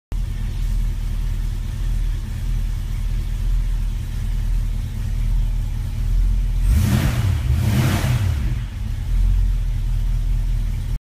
November 1, 2017
Marshall gauge install in a Scott Drake instrument panel.